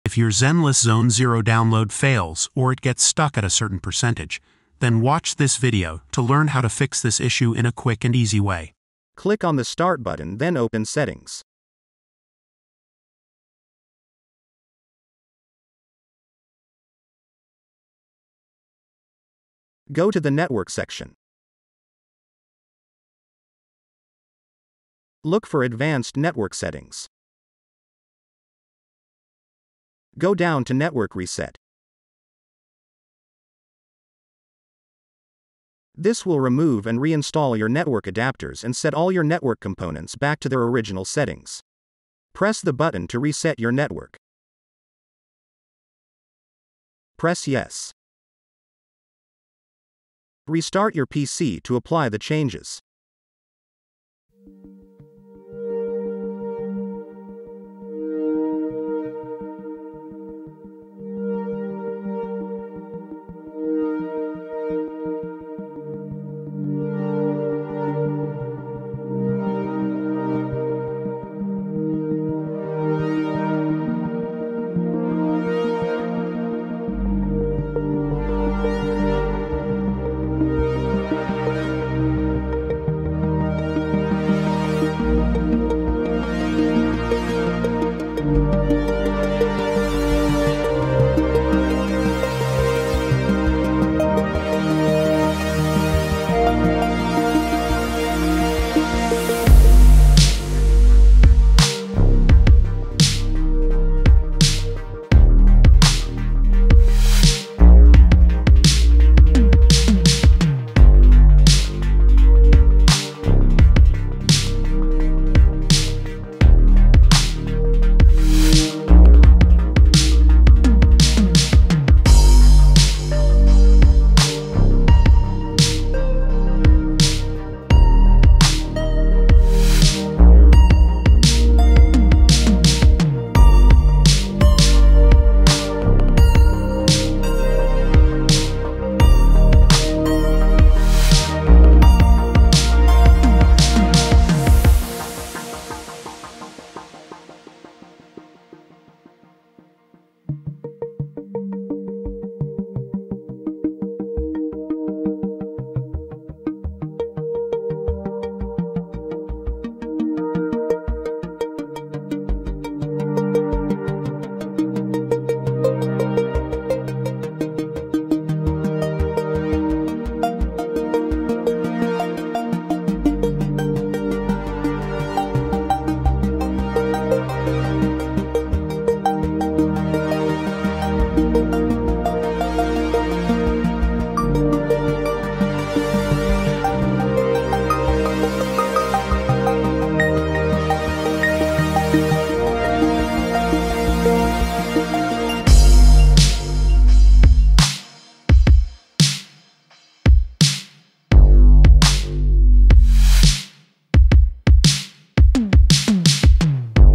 How to Fix Zenless Zone Zero download failed 200 - Download Stuck
In this video, we'll show you how to fix the "Download Failed" error in the Epic Games Store. Follow our step-by-step guide to resolve this frustrating issue and get back to enjoying your games.

 》 CHAPTERS 《

00:00 Intro
00:09 Solution 1
01:07 Solution 2
02:16: Solution 3
03:20: Outro